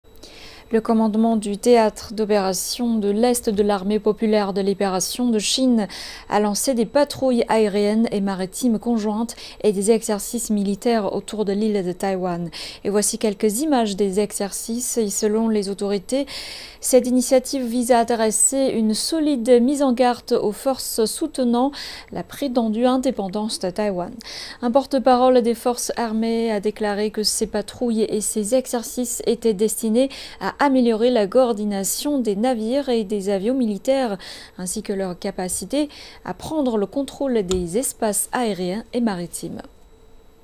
L'APL lance des patrouilles conjointes et des exercices militaires autour de Taiwan
Le commandement du Théâtre d'opérations de l'est de l'Armée populaire de libération (APL) a lancé samedi des patrouilles aériennes et maritimes conjointes, ainsi que des exercices militaires de la marine et de l'armée de l'air autour de l'île de Taiwan.
Ces patrouilles et exercices ont pour but d'entraîner la coordination des navires et des avions militaires, ainsi que leur capacité à prendre le contrôle des espaces aériens et maritimes, a déclaré Shi Yi, porte-parole du commandement.
La capacité des forces armées à combattre dans des conditions réelles sera testée, a indiqué M. Shi, ajoutant que les patrouilles et exercices constituaient un avertissement sévère à la collusion entre les séparatistes recherchant "l'indépendance de Taiwan" avec des éléments étrangers, et à leurs provocations.